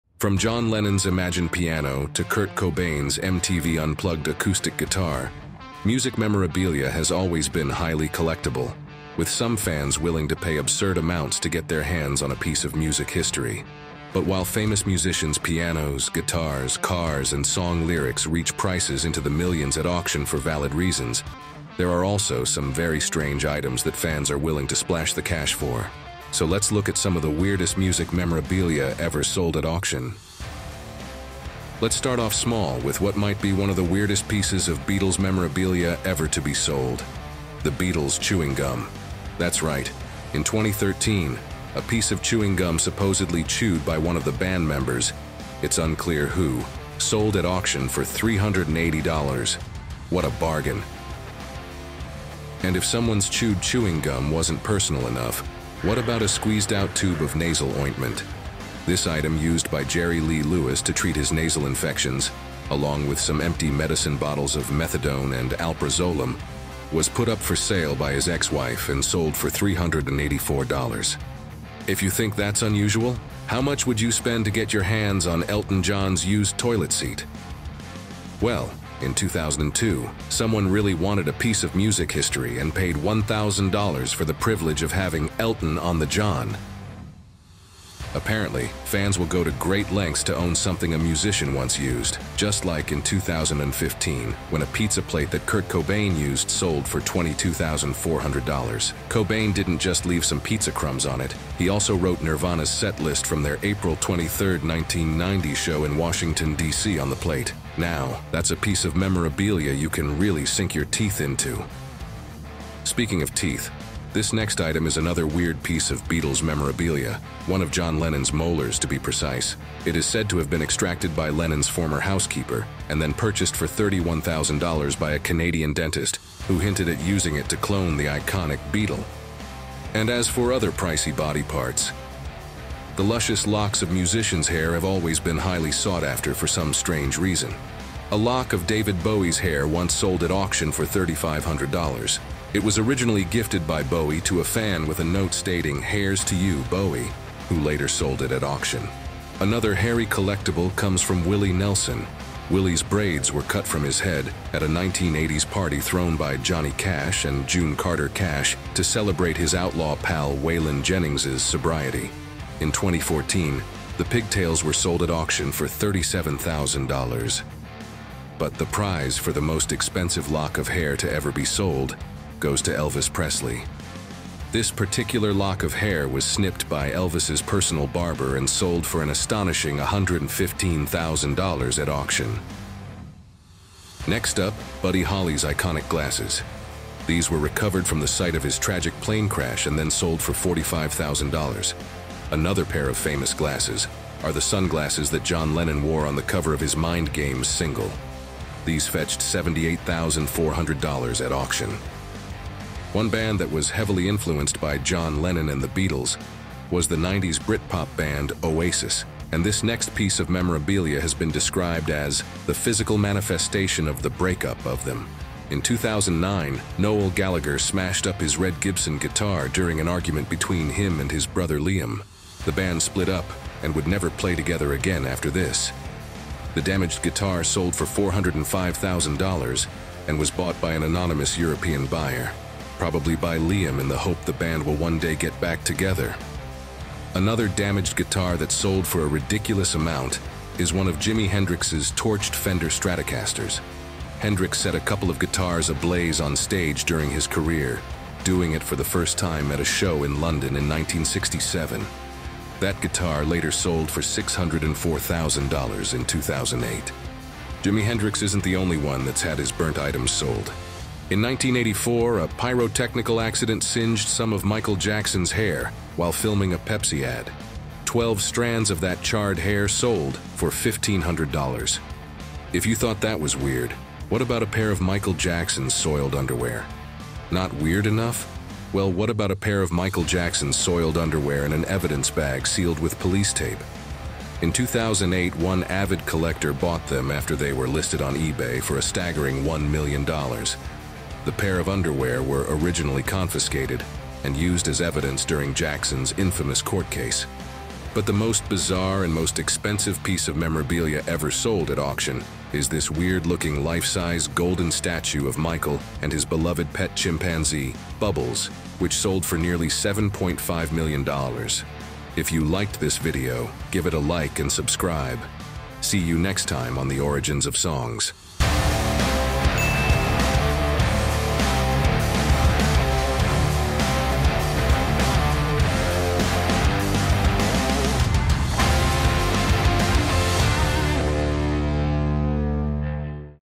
The Weirdest Music Memorabilia Ever Sold At Auction! (Crazy Music Facts)
Music memorabilia can fetch enormous sums at auction, with fans willing to pay for both iconic and unusual items. While famous instruments and personal belongings of musicians are expected to be valuable, some of the weirdest music memorabilia sold include a piece of chewing gum supposedly chewed by a Beatle, John Lennon’s molar, and Michael Jackson's soiled underwear. But for how much??

From the channel that brings you crazy music facts.